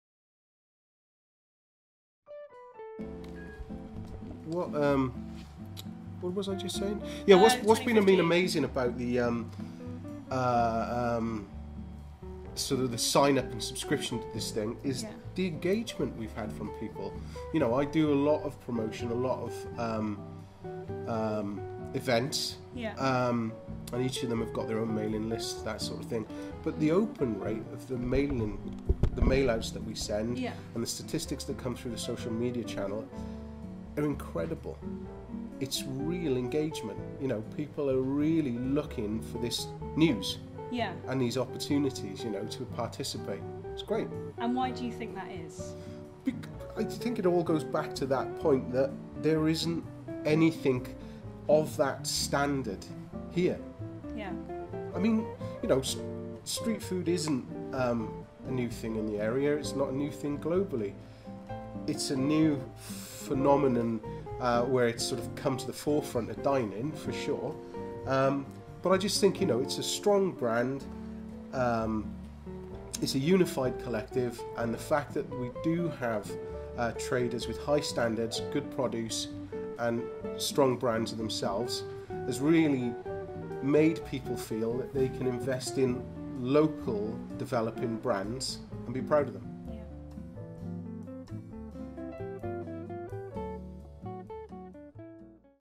Simon Thomas on Street Feast Cardiff
Street Feast Cardiff has bees the biggest pop up event of the season in
the Welsh capital. Organizer Simon Thomas talks to Eat Happy about its
success.